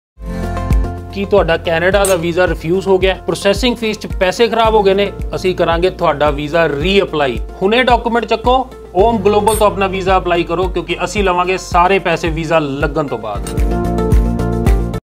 Invitation to Aum Global's "Study In Canada" Seminar
We are inviting Sept Intake refused students to apply for NovIntake.
With Aum Global
𝗦𝗲𝗺𝗶𝗻𝗮𝗿 𝗱𝗮𝘁𝗲𝘀: 1st Sept to 4th Sept
𝗞𝗮𝗿𝗻𝗮𝗹 | 𝗖𝗵𝗮𝗻𝗱𝗶𝗴𝗮𝗿𝗵 | 𝗝𝗮𝗹𝗮𝗻𝗱𝗵𝗮𝗿 | 𝗔𝗺𝗿𝗶𝘁𝘀𝗮𝗿